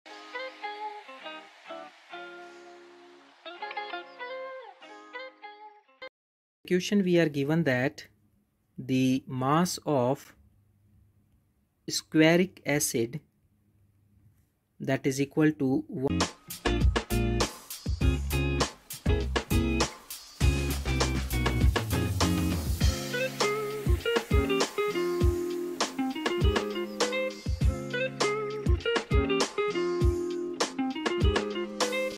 The organic compound squaric acid, shown here, is useful for preparing a variety of dyes. Determine…
The organic compound squaric acid, shown here, is useful for preparing a variety of dyes. Determine the number of moles of squaric acid in 113.2 g mol C4H4O4.